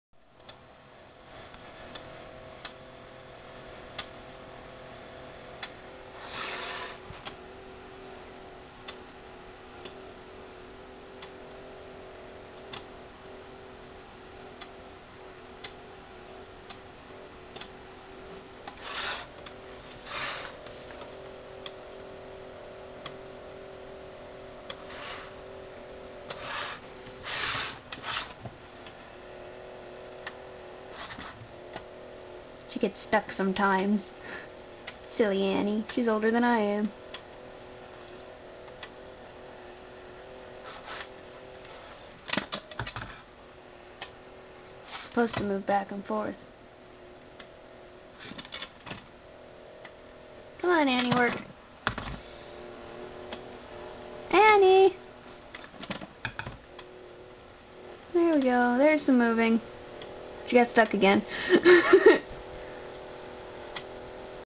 swim annie swim
Lol an old fish toy I own that I still Love- We call her annie-
Go annie go!